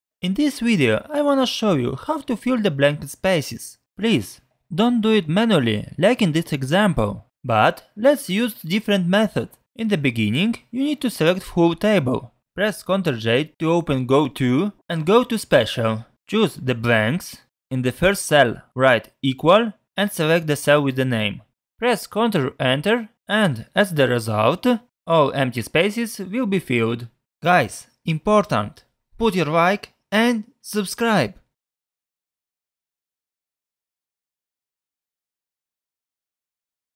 How to Fill Blank Spaces in Excel
Description:
📊 Frustrated with empty cells and blank spaces in your Excel data? In this step-by-step Excel tutorial, How to Fill Blank Spaces, we'll guide you through the process of filling those gaps, enhancing data completeness and clarity Fill Blank Spaces in Excel,. Say goodbye to incomplete spreadsheets and hello to seamless data! 📈✍️

✒️ Learn essential techniques for filling blank spaces, explore Excel functions like IF(), CONCATENATE(), and more, and discover time-saving tips for data completeness and accuracy. Boost your spreadsheet skills and ensure your data is ready for analysis and reporting. ⏱️

👨‍💻 No advanced Excel skills required Fill Blank Spaces. We'll provide clear instructions and practical examples to help you become an Excel pro in data filling How to Fill Spaces in Excel. Optimize your data management and never leave blank spaces behind Fill Spaces in Excel! 📊✨